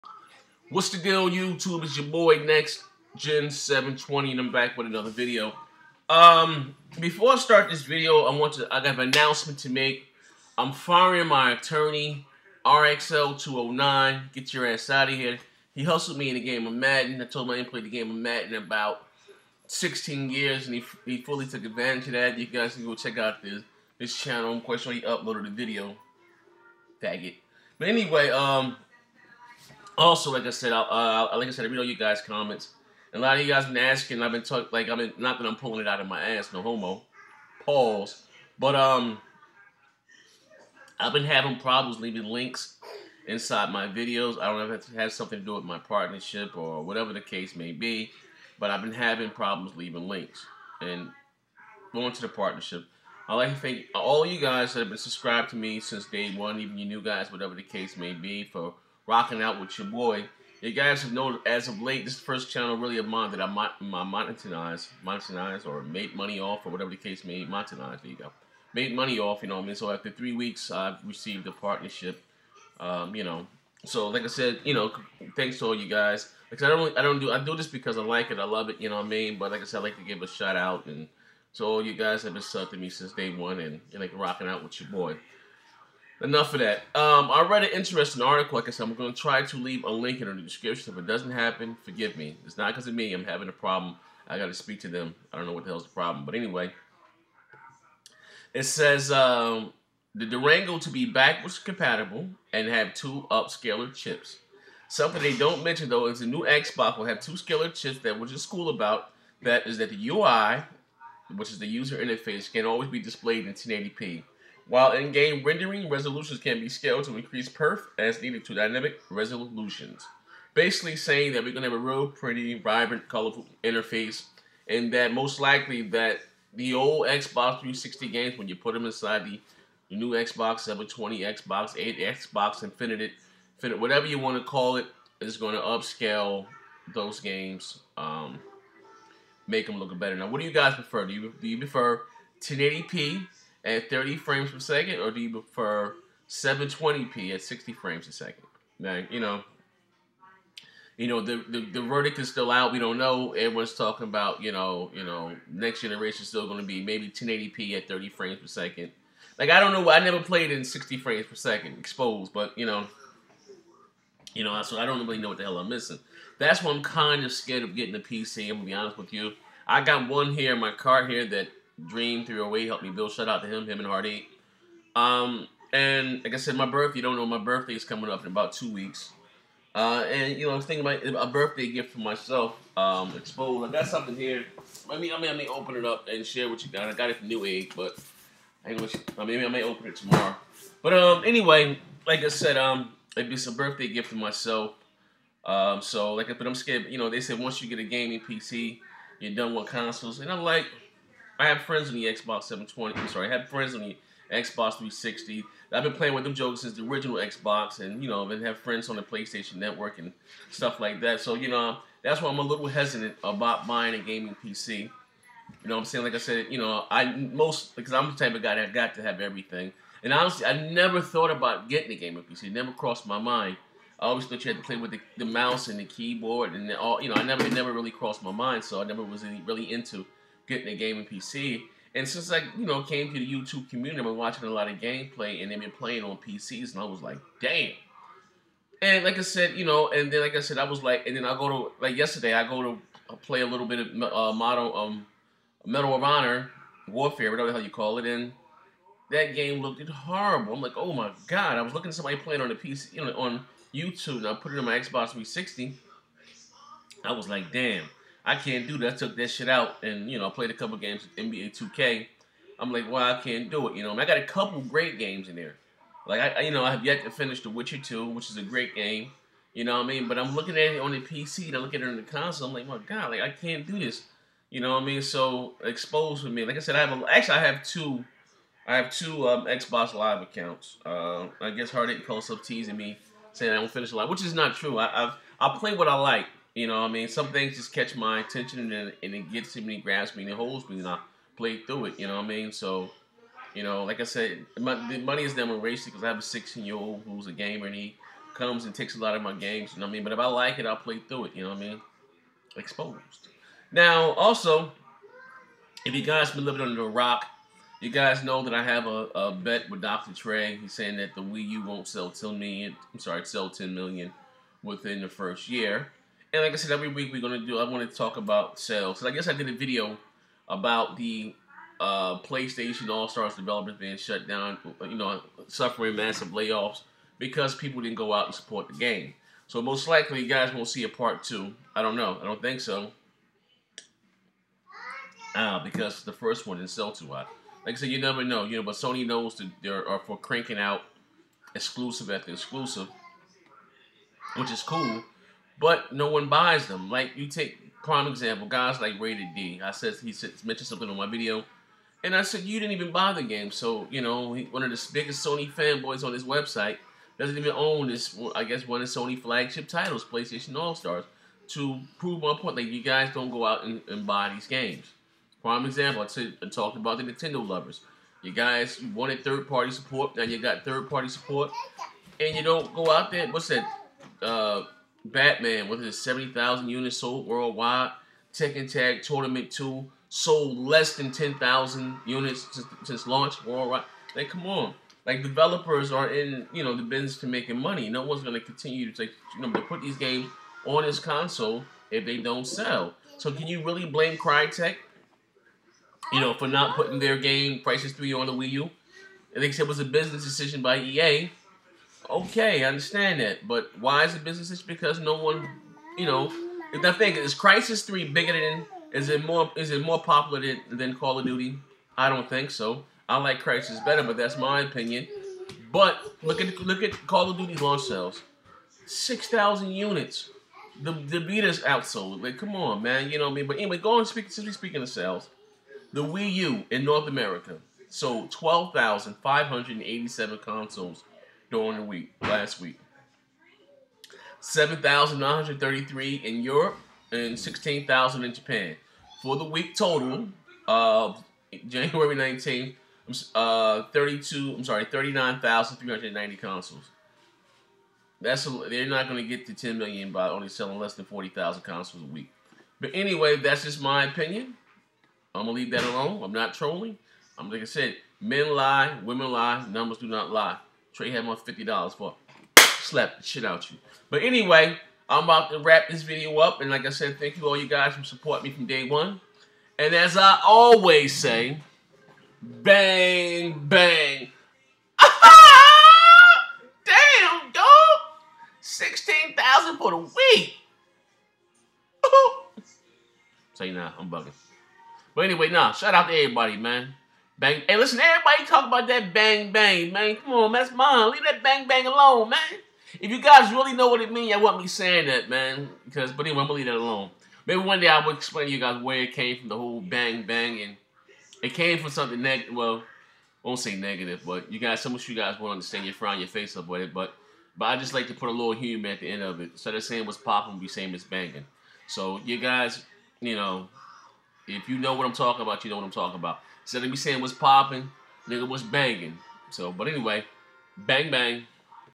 XBOX ONE WILL HAVE 2 UPSCALER CHIPS
ACCORDING TO RUMORS THE 720 WILL HAVE 2 UPSCALER CHIPS